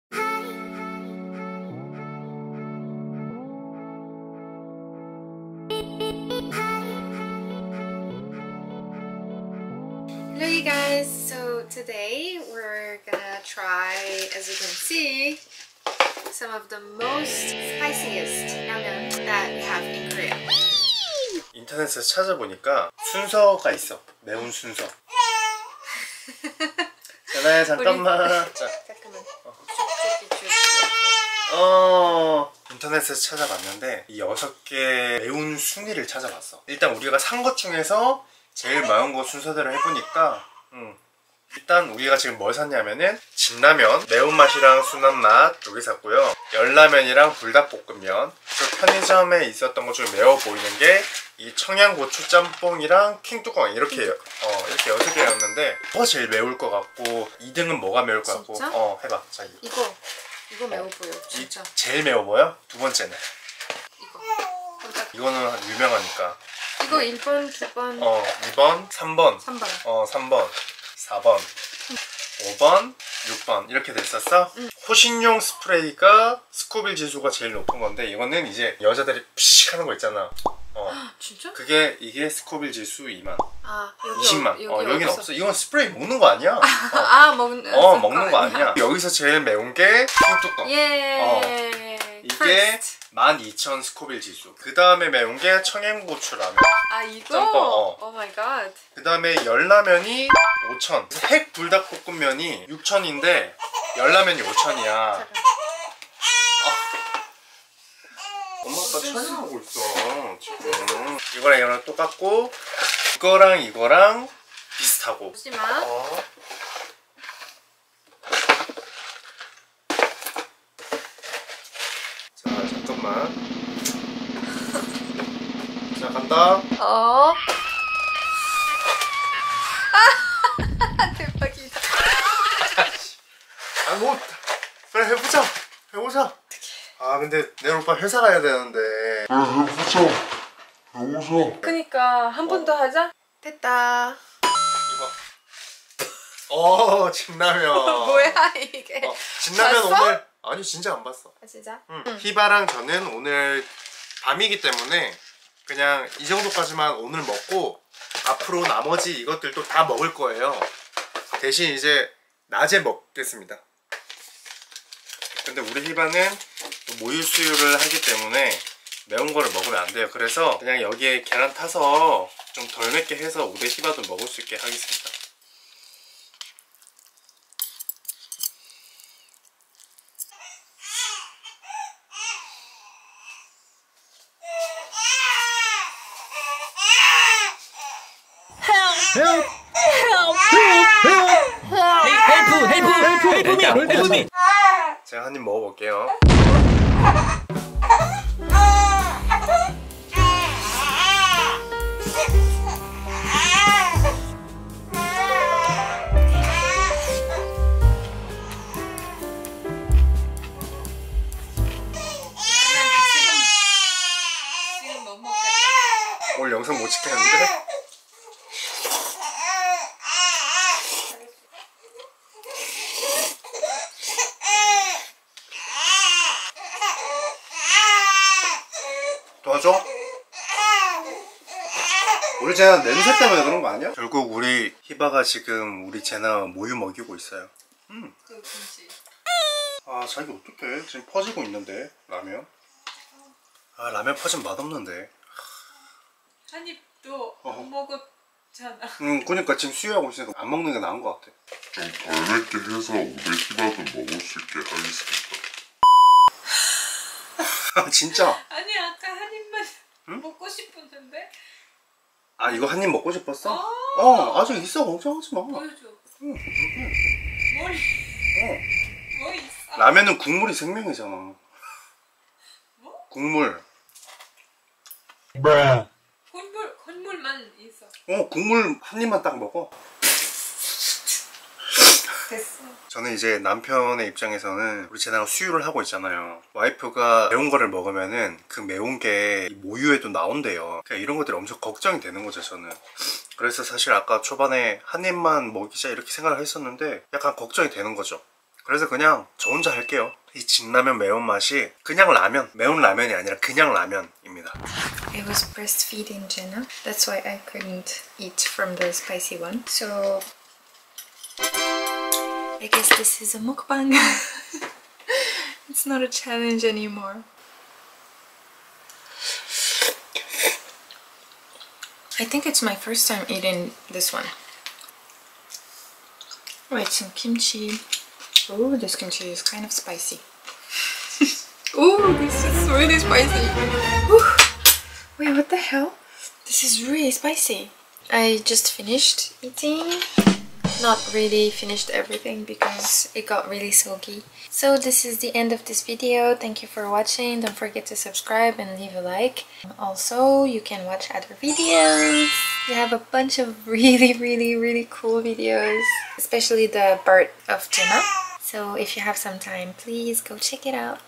[SUB] Trying to film a spicy ramyeon challenge with a baby 🤧💀 *IMPOSSIBLE*
💖Caption 💖
Hello Besties,

We wanted to do this spicy ramyeon challenge but reality hit us quickly when we faced our baby's cries. This is just a little sneak peak into how difficult it becomes, to do anything for yourself outside of caring for your baby. But we truly feel blessed! We take this experience with smiles and laughs and adapt to our new life. Thank yall for the support, always ~ Love.
(If you have any questions don't hesitate. I've been receiving quite a few messages on my instagram, its always a pleasure to read you all.)